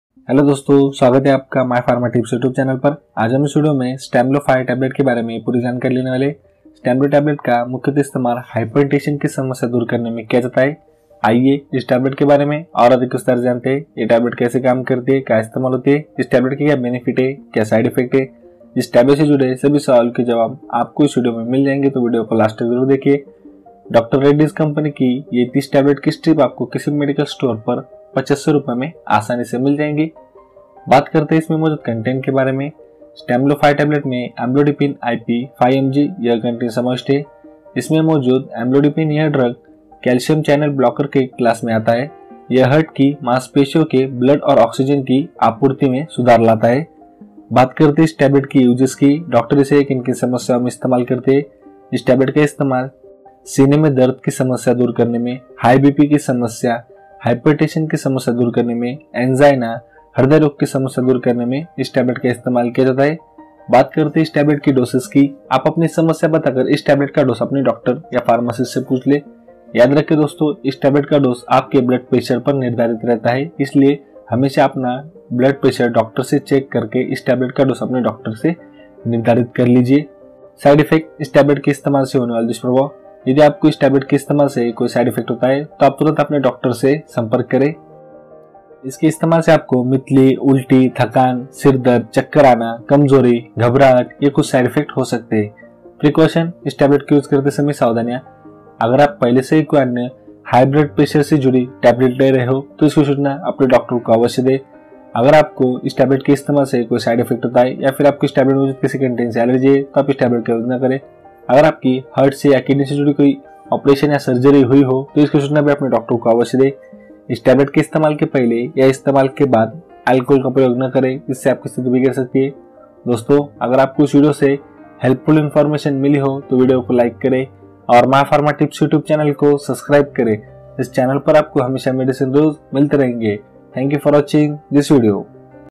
stamlo 5 mg tablet uses | price | composition | dose | side effects | precautions | in hindi
This video is all about stamlo 5 mg tablet, its uses, side effects, ingredients, price, dose, precautions and how to take, full detail in Hindi